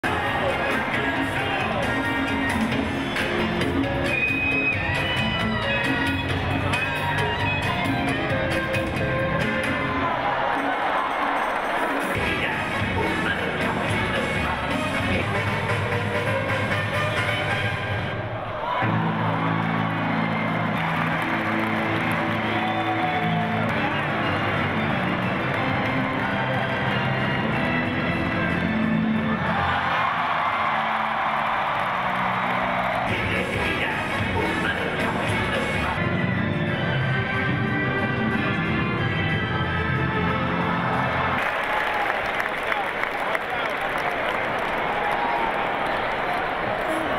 Quick Change Artists Dazzle haltime crowd at Northern Iowa
David and Dania do some quick costume change you have to see